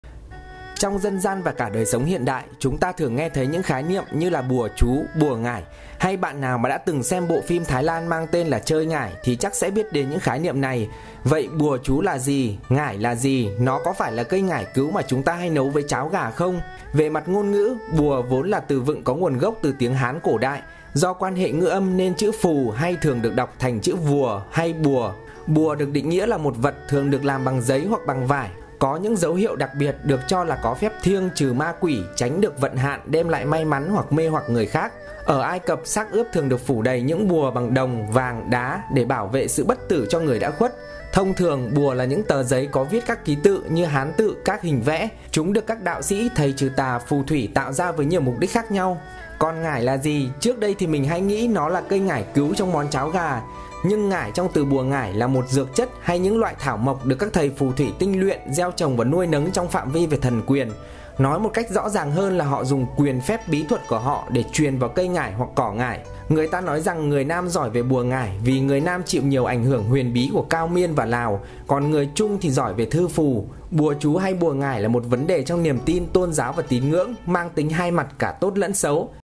Bùa chú và bùa ngải là gì?
Về mặt ngôn ngữ, bùa vốn là từ vựng có nguồn gốc từ tiếng Hán cổ đại. Do quan hệ ngữ âm nên chữ Phù hay được đọc thành vùa hay bùa. Bùa được định nghĩa là một vật thường được làm bằng giấy hoặc bằng vải, có những dấu hiệu đặc biệt được cho là có phép thiêng trừ ma quỷ, tránh được vận hạn, đem lại may mắn hay mê hoặc người khác. Ở Ai Cập xác ướp được phủ đầy những bùa bằng vàng, đồng, đá… để bảo vệ sự bất tử cho người đã khuất. Thông thường bùa là giấy có viết các ký tự như Hán tự, các hình vẽ. Chúng được các đạo sĩ, thầy trừ tà, phù thủy tạo ra với nhiều mục đích khác nhau.